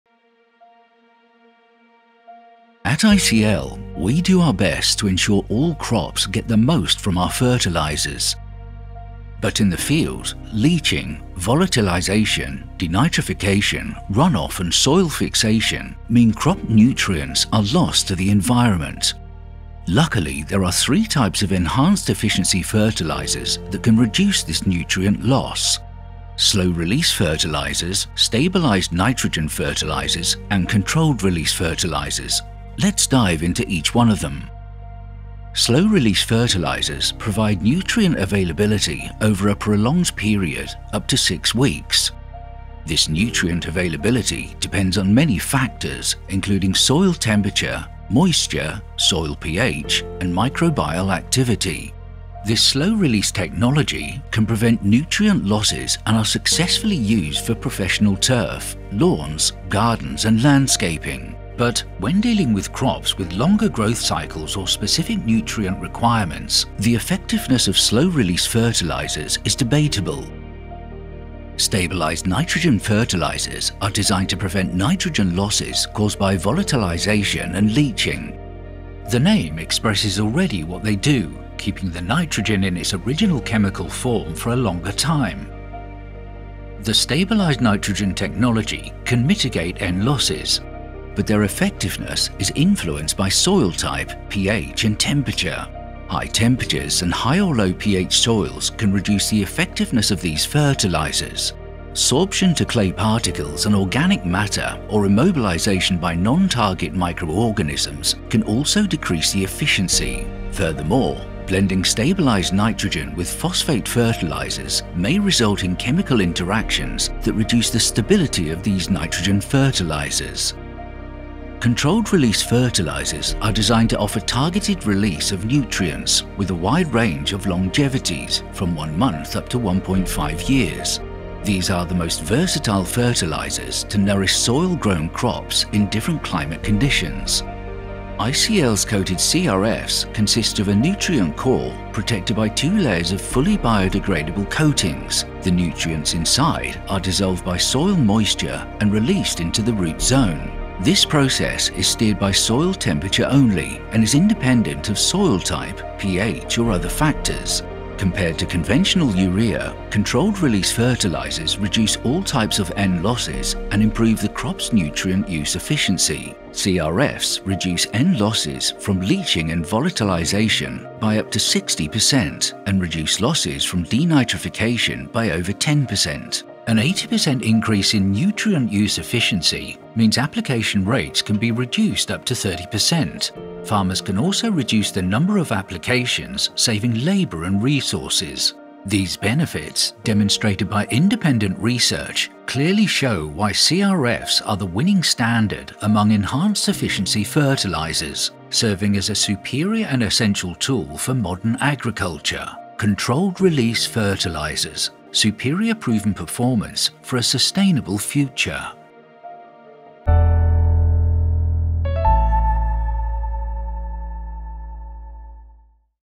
ICL's Controlled Release Fertilizers for Superior Performance
The benefits of using fertilizers are undeniable. Investing in buying and applying fertilizers provides the nutrients crops need for healthy growth and improved harvests. However, with every fertilizer application, there is the chance that some of the nutrients will be lost to the atmosphere or deep into the soil beyond the reach of the crops’ roots before the plants can effectively utilize them.

Of the three types of enhanced-efficiency fertilizers available to reduce these nutrient losses, controlled-release fertilizers (CRF) are the most sophisticated, offering targeted release of nutrients at rates that match the crops’ requirements. With longevities of between 1 month and 1.5 years, ICL’s controlled-release fertilizers are the most versatile fertilizers for nourishing soil-grown crops, whatever the climate conditions.

Find out how these controlled-release fertilizers compare to conventional and other enhanced-efficiency fertilizers, reducing all types of nitrogen losses and improving the nutrient use efficiency of crops, meaning that growers can reduce application rates by up to 30%.

With benefits like this, demonstrated by independent research, it is clear that CRFs are the winning standard among enhanced-efficiency fertilizers, serving as a superior and essential tool for modern agriculture.